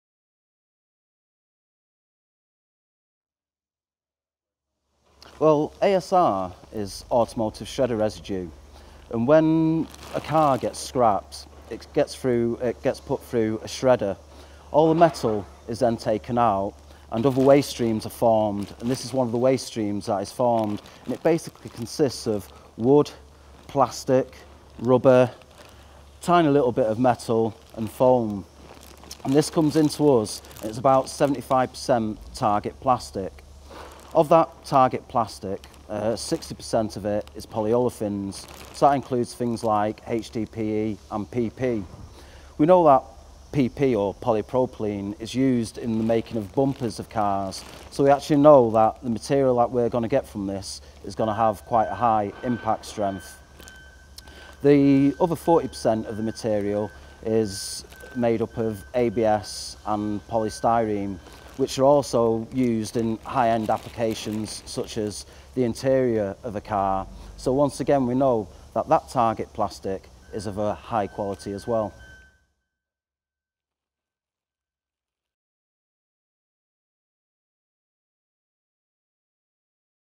Recycling Plastics - The source material - ASR
Peter Mackrell, Quality Control Manager at MBA Polymers, talks about Automotive Shredder Reside - ASR - and its place in plastics recycling